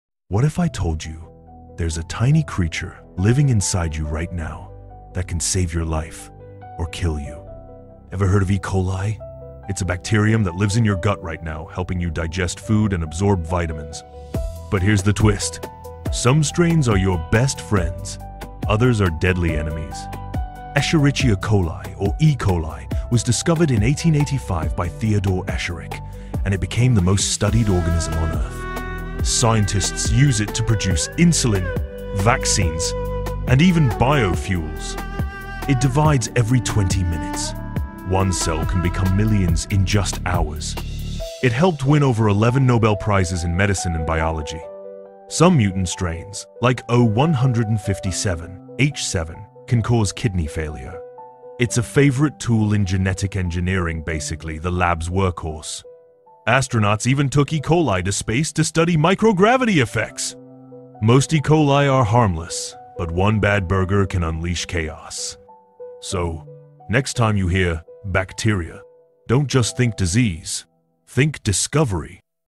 Friend or Foe: E. Coli
Enter into the microscopic world of E.coli and learn fascinating facts about this bacteria that has been a sensation in the world of medicine and biology.

Have fun learning new and inetersting concepts and theories about economics, science and history.